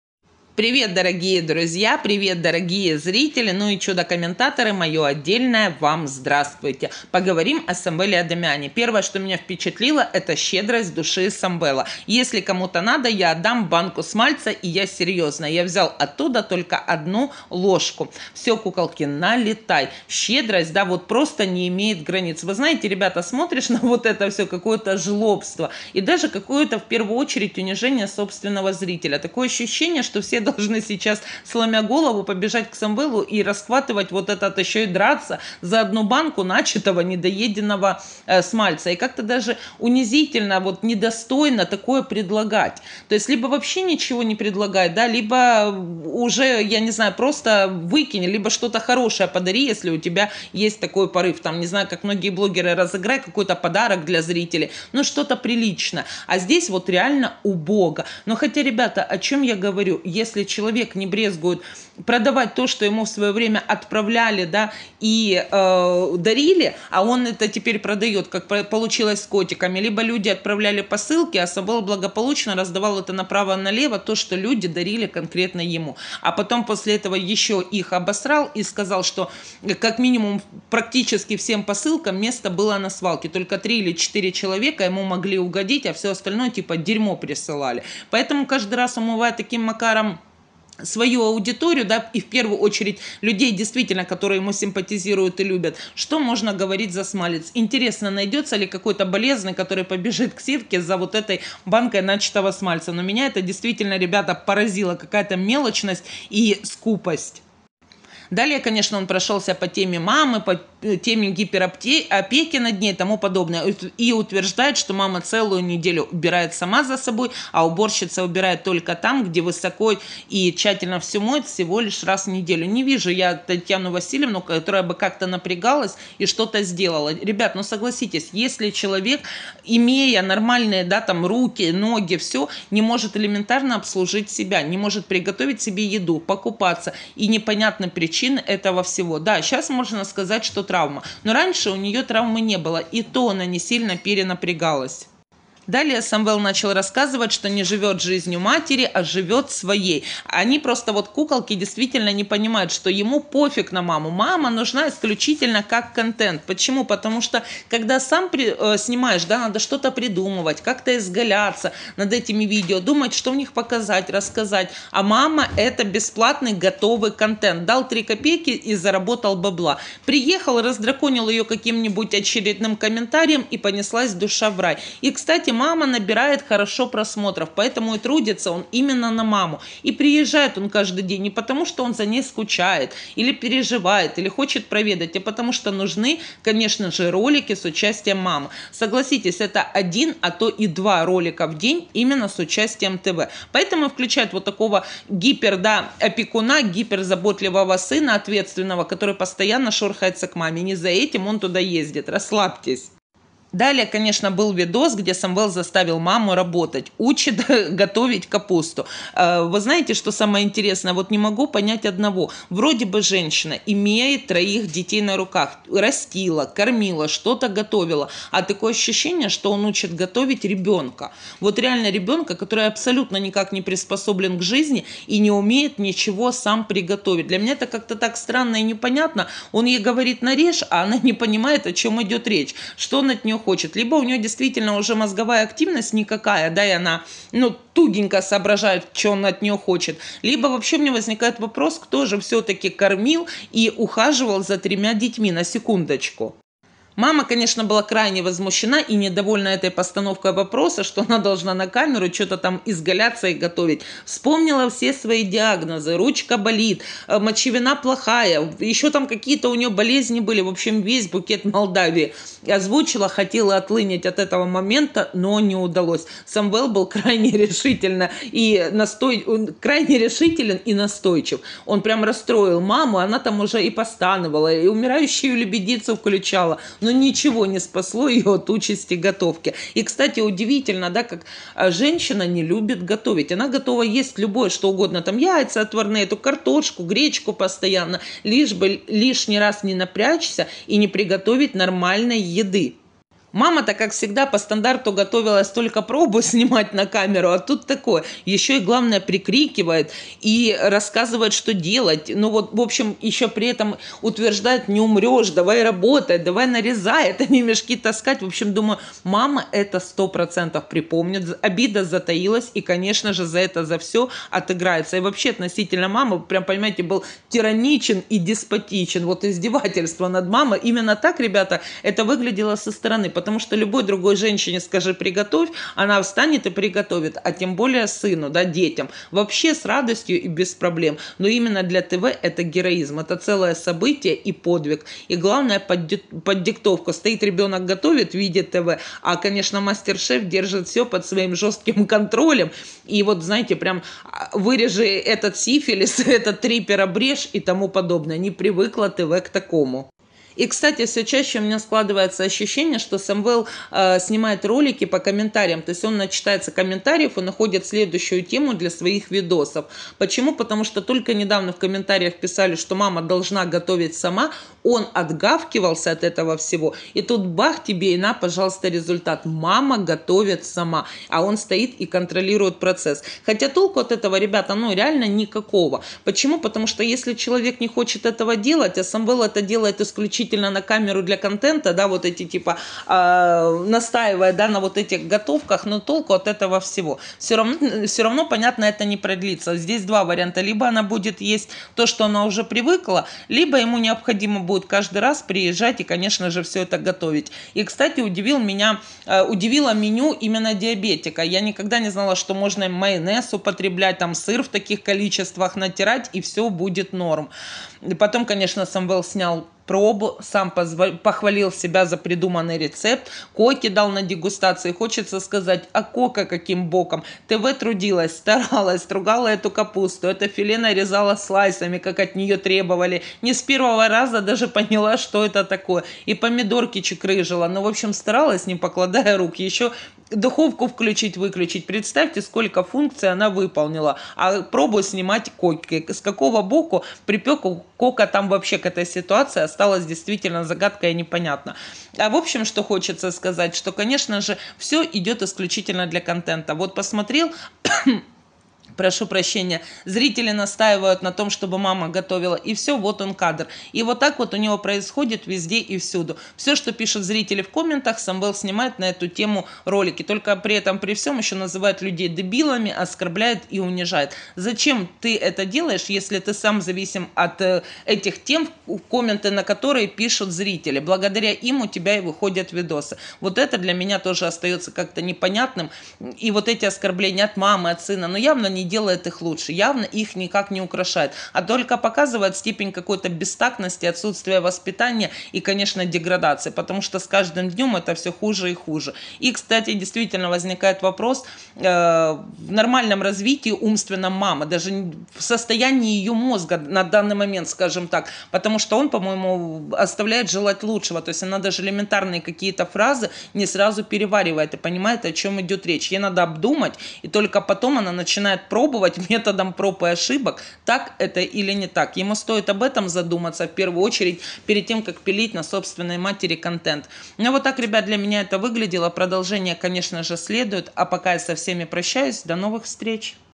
Самвел Адамян тиран и деспот оскорбляет мать \ Saveliy Ad, семья Адамян, grandenikо vlog
Привет, дорогие друзья. Вы на канале - Правдивая Ольга. Сегодня хочу поговорить о Самвеле, Эдике и grandenikо vlog и маме, в общем о семье Адамян.